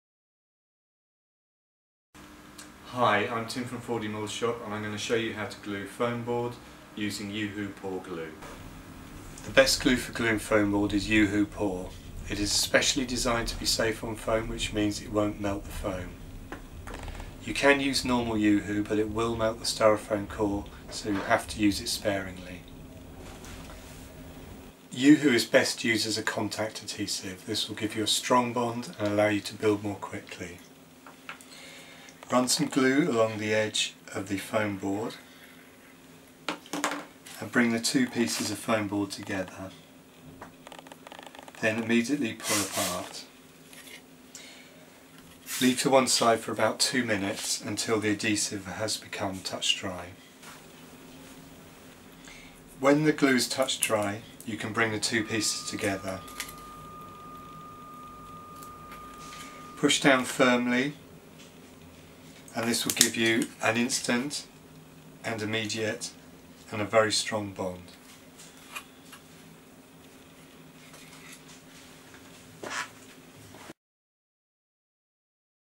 Gluing foamboard
Traditionally used for mounting artwork, foamboard great model making and architectural sketch modelling material.

Printed elevations can be spraymounted onto the surface and cut out with a scalpel and safety ruler.

Our guide to gluing foamboard.